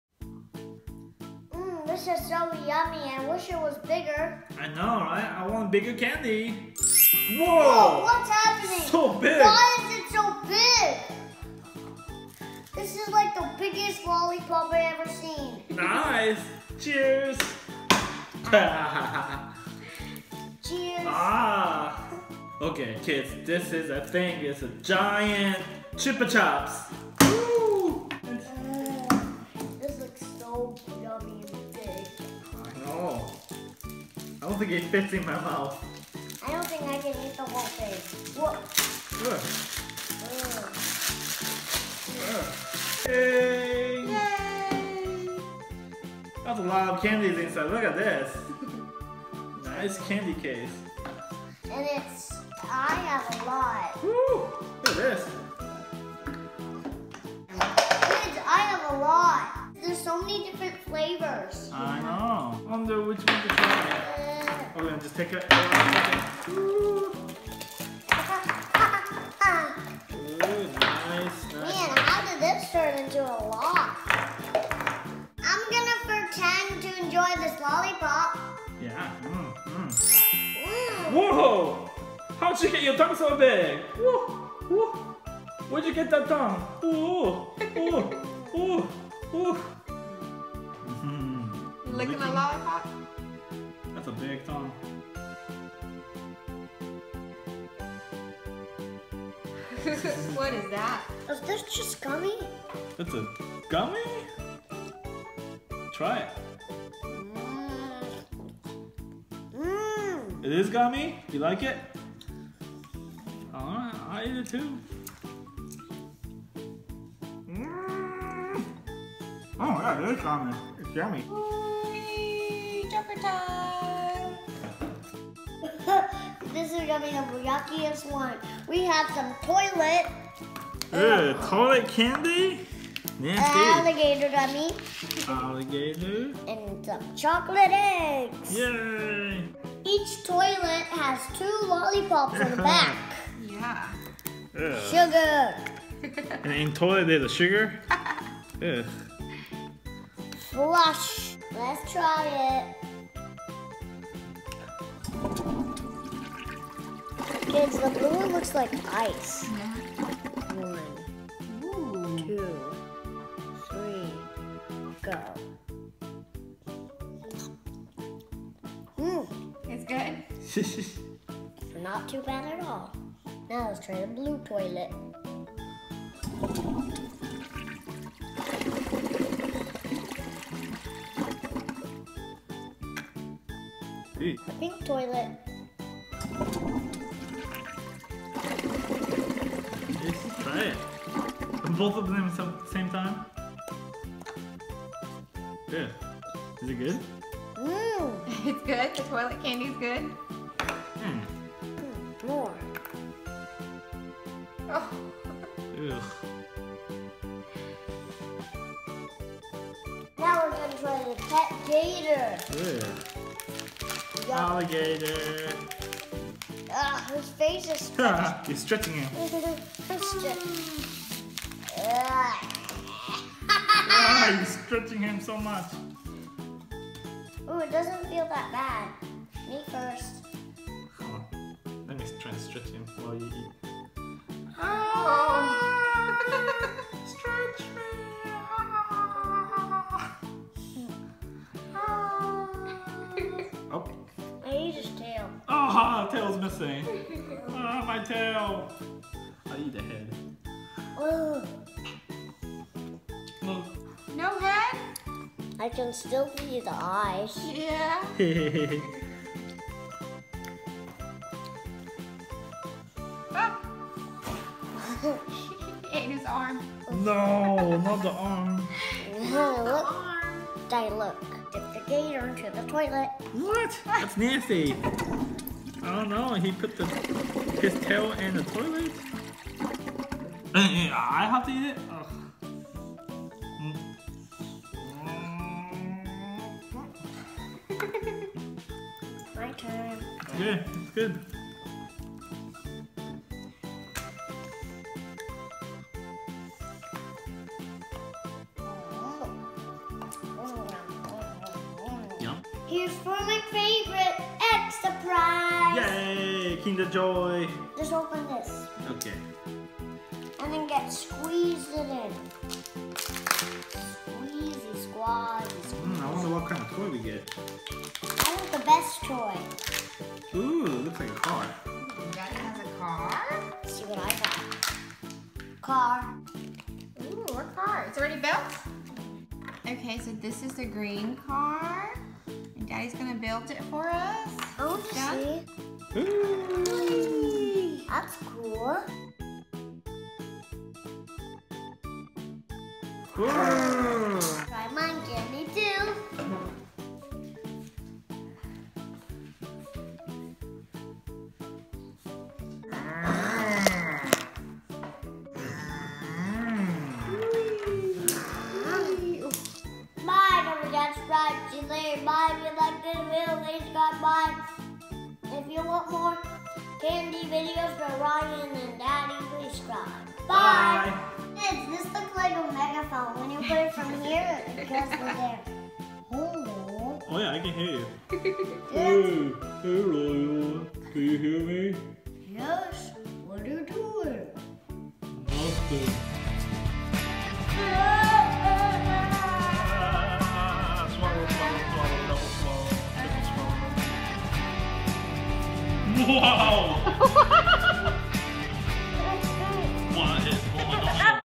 Giant Chupa Chups Lollipops Candy Review
Join Ryan ToysReview as we do another Candy Review and taste Giant Chupa Chups Lollipops, Gummy Joker Tongue, Toy Toilet Candy Sour Flush , Gummy Gator Pet, and open Kinder Eggs Surprise Toys! Watch to see which surprise toys we got out of the chocolate eggs and which candies were Ryan's favorite!